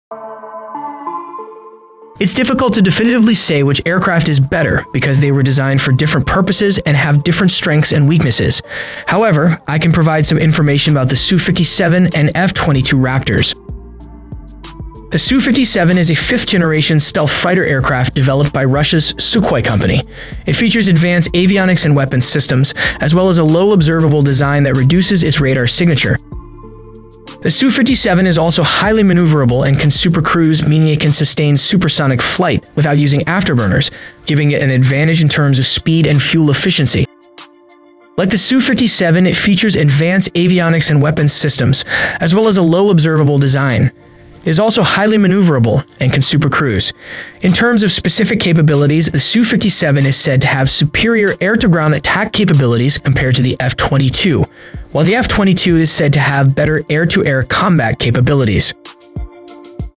Su-57 is More Advanced than F-22 Raptor, So Why is it better?
It's difficult to definitively say which aircraft is "better" because they were designed for different purposes and have different strengths and weaknesses. However, I can provide some information about the Su-57 and F-22 Raptors.